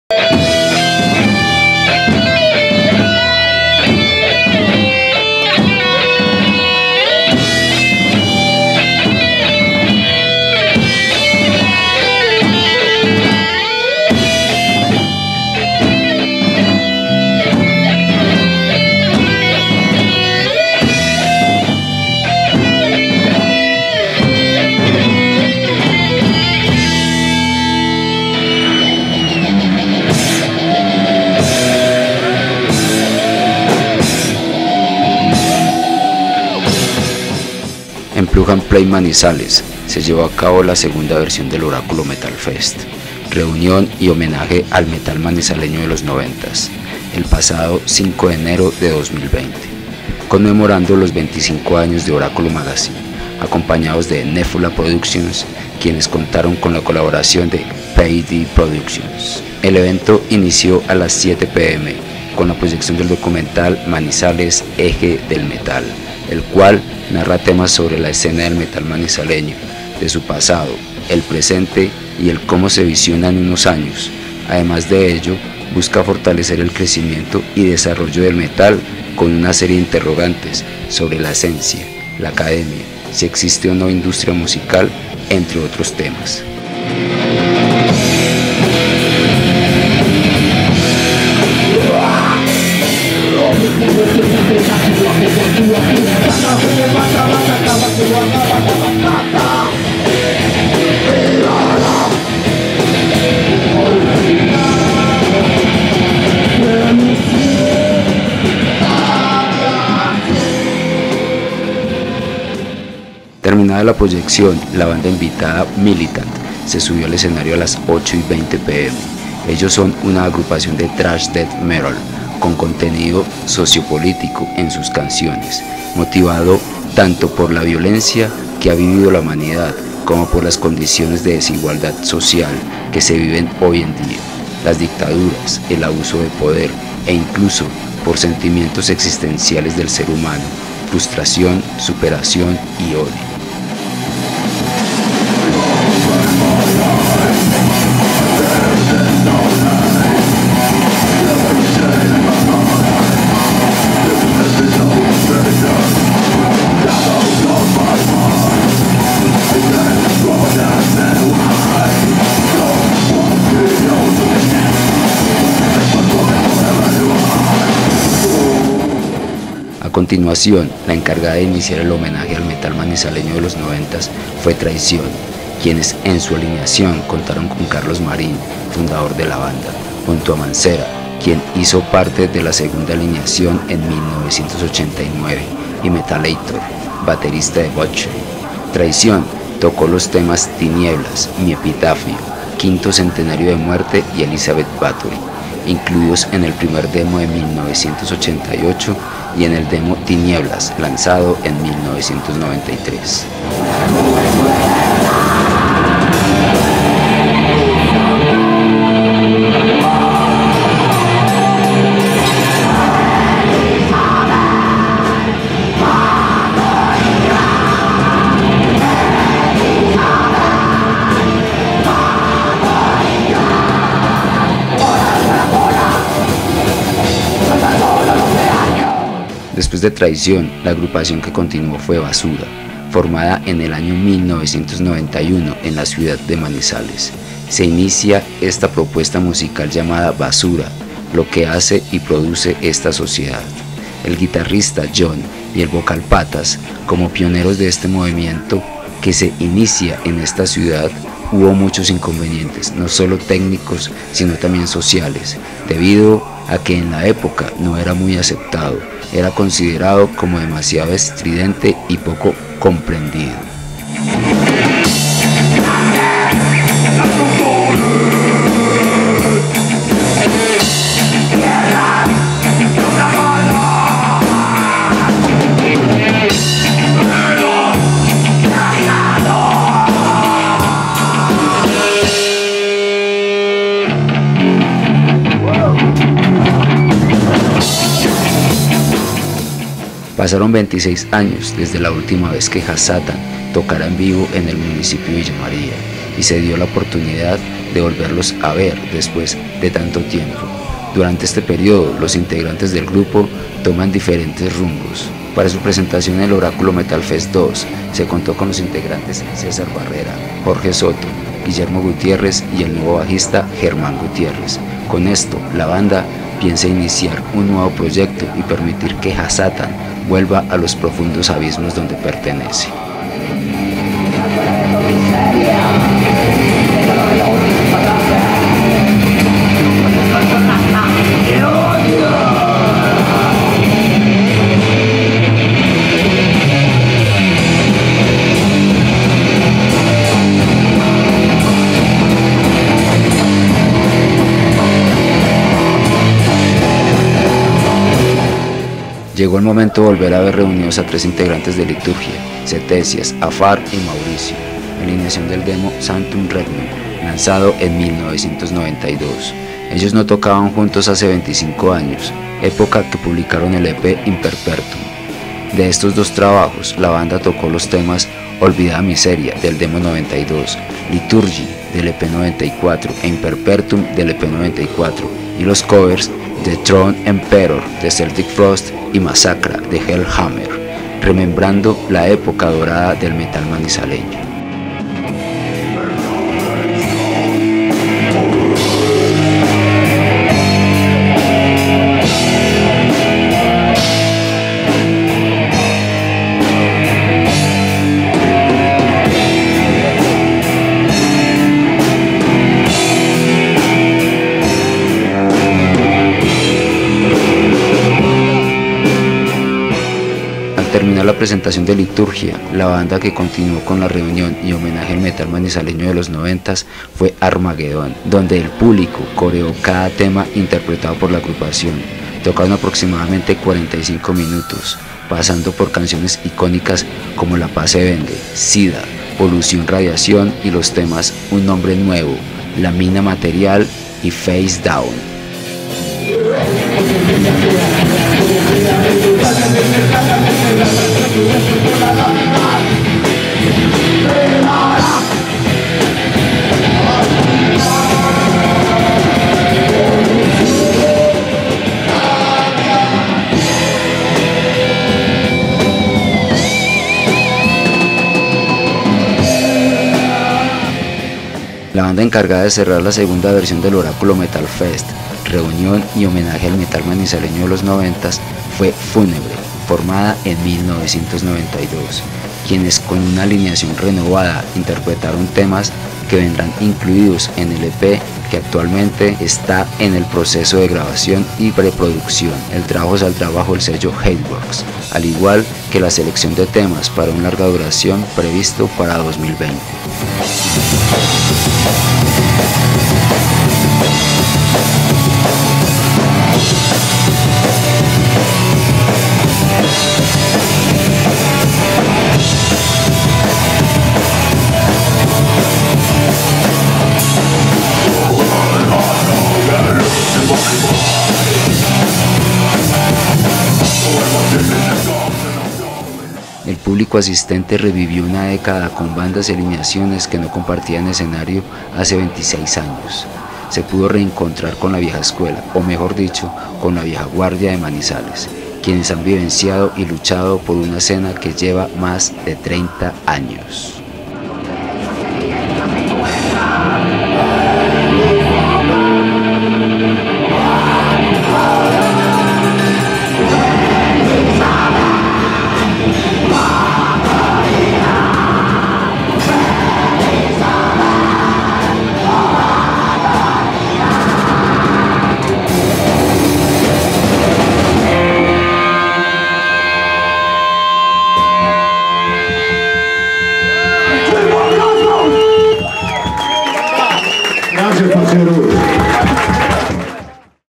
Oráculo Metal Fest II - Video Reseña
Reunión y Homenaje al Metal Manizaleño de los 90, Conmemoración 25 Años de Oráculo Magazine
05 de Enero de 2020
Manizales

Organizado por; Oráculo Magazine, Nephula Productions

Bandas: Militant, Traición, Basura, Hasssatan, Liturgia, Armagedon, Fúnebre